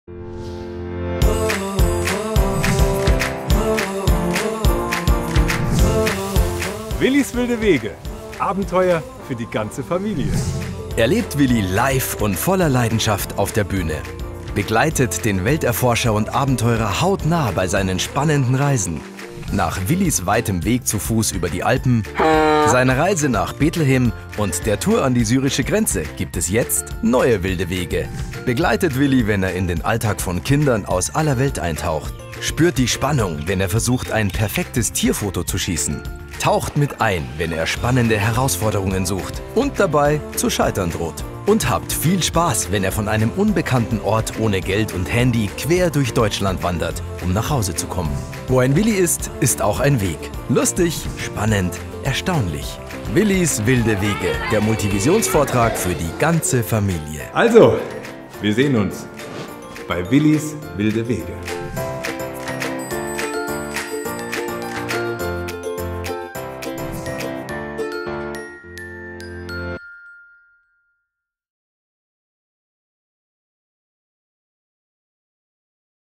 Willis Wilde Wege LIVE am 17.10.2021 in der Stadthalle Gersthofen
Willi Weitzel wurde mit seiner Sendung auf kika zum Medienstar bei den Kids und ihren Eltern. am 17. Oktober könnt ihr ihn nun live in der stadthallegersthofen erleben.